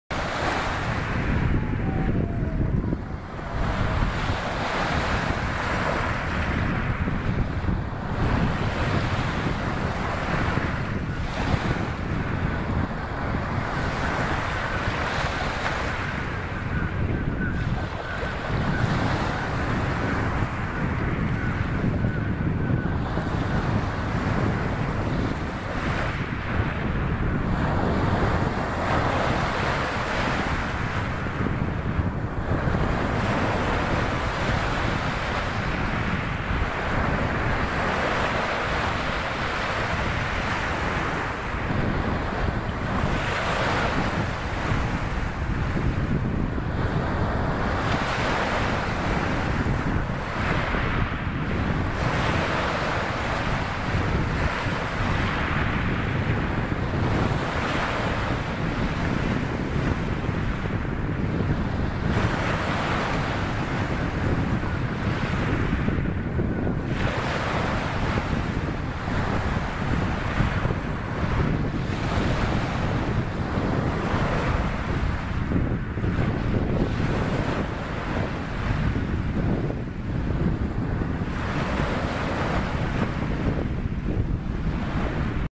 markaczka / Common Scoter / Melanitta nigra, Baltic Sea coast, Poland, April 2018
Common Scoter courtship